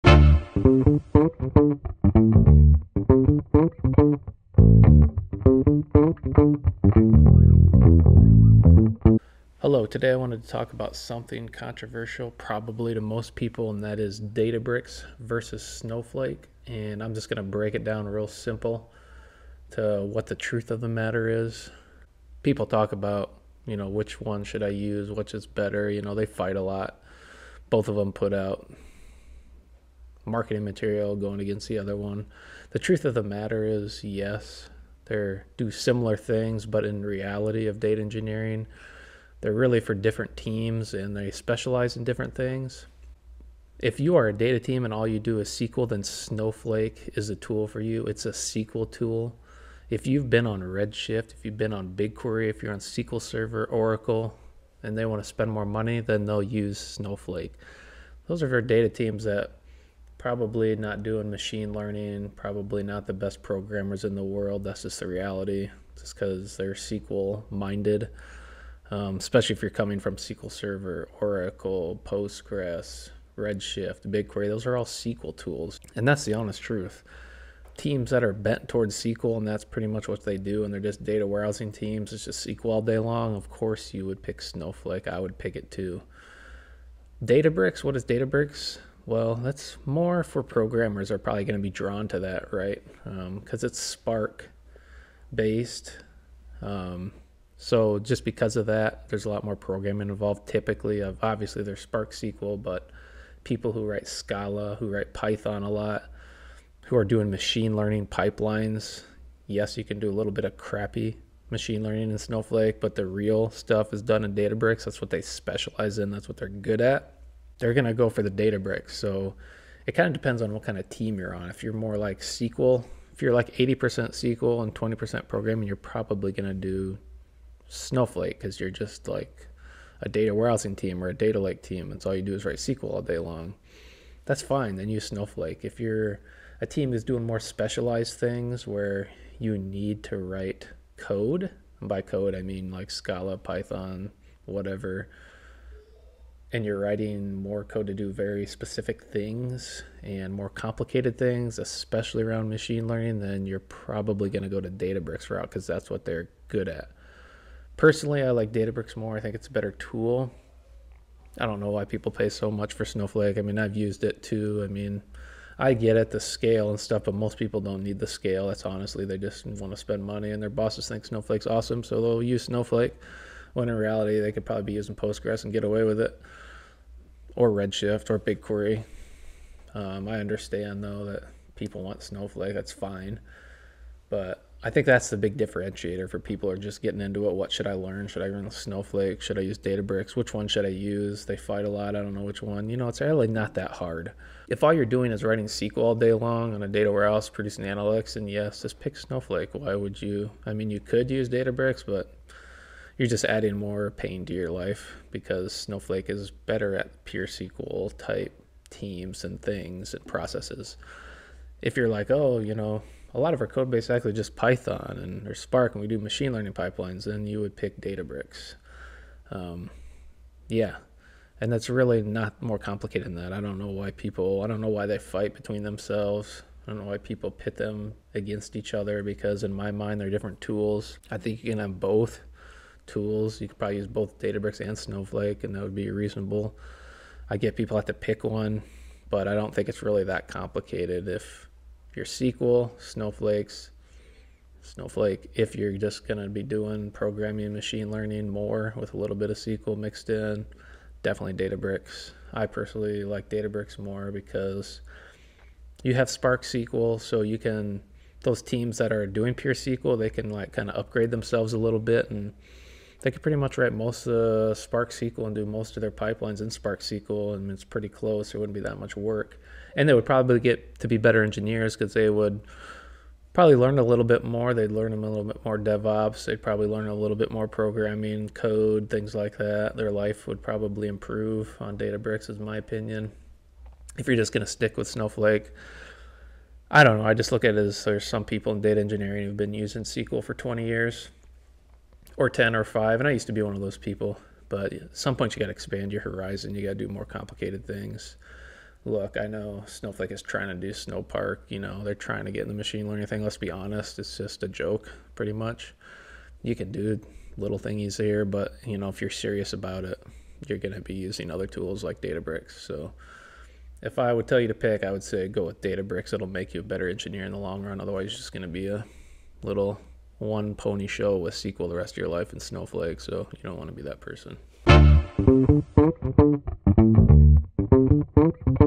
Snowflake vs Databricks
What is the difference between snowflake and databricks? What should a data team pick? The Average Engineer tells all.